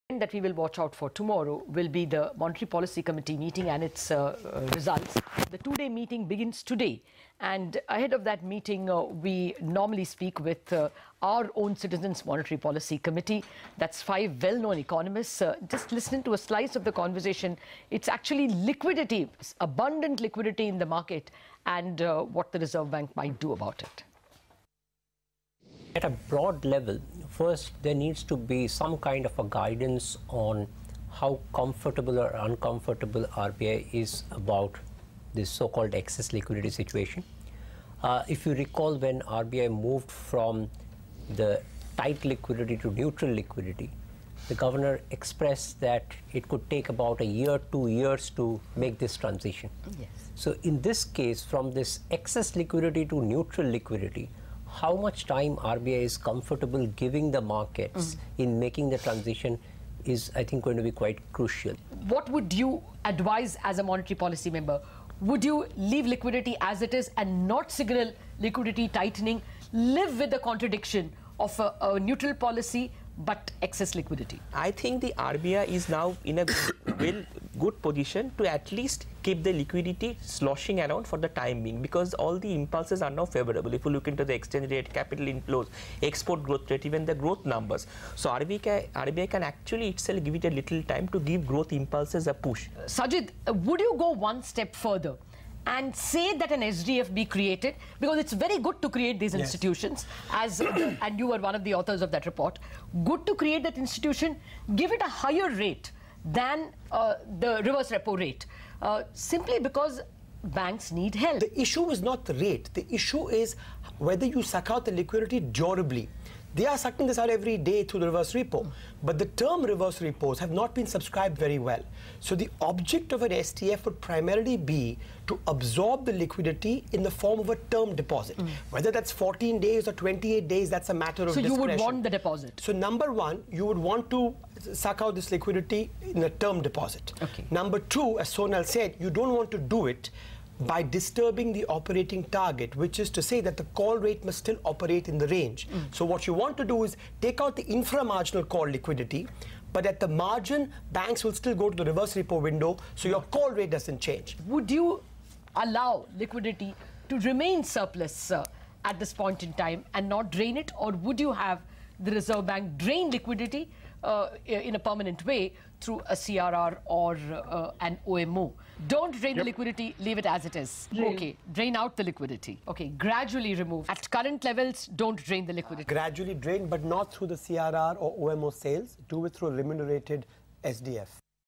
Monetary Condition In India Today Is At A 2-Year Low: Citi
Samiran Chakraborty, Chief Economist, Citi states that the monetary condition in India is at a 2-year low.